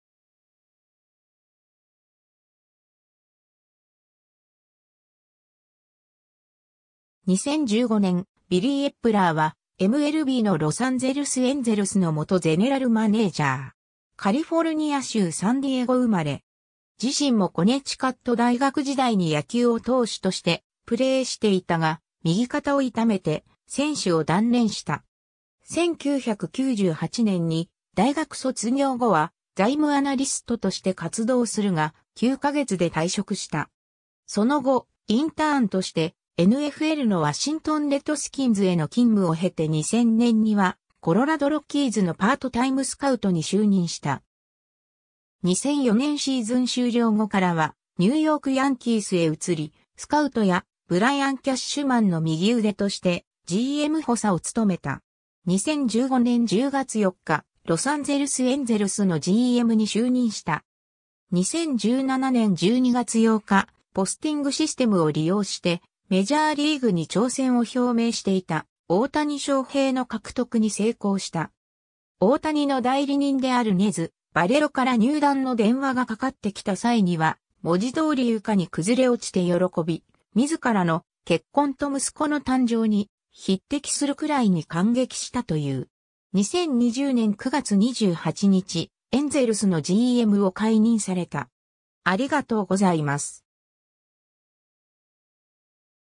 ビリー・エップラー
ワシントン・レッドスキンズ関連人物
コロラド・ロッキーズ関連人物
ニューヨーク・ヤンキース関連人物
ロサンゼルス・エンゼルス関連人物
2015年 ビリー・エップラー（Billy Eppler 1975年9月16日 - ）は、MLBのロサンゼルス・エンゼルスの元ゼネラルマネージャー （GM）（2015年10月 - 2020年9月）。
 カリフォルニア州サンディエゴ生まれ。
自身もコネチカット大学時代に野球を投手としてプレーしていたが、右肩を痛めて選手を断念した。
 1998年に大学卒業後は財務アナリストとして活動するが、9ヶ月で退職した。
その後、インターンとしてNFLのワシントン・レッドスキンズへの勤務を経て2000年にはコロラド・ロッキーズのパートタイムスカウトに就任した。
2004年シーズン終了後からはニューヨーク・ヤンキースへ移り、スカウトやブライアン・キャッシュマンの右腕としてGM補佐を務めた。
 2015年10月4日、ロサンゼルス・エンゼルスのGMに就任した。
 2017年12月8日、ポスティングシステムを利用してメジャーリーグに挑戦を表明していた大谷翔平の獲得に成功した。
大谷の代理人であるネズ・バレロから入団の電話がかかってきた際には「文字通り床に崩れ落ちて」喜び、自らの「結婚と息子の誕生に匹敵」するくらいに感激したという。
 2020年9月28日、エンゼルスのGMを解任された。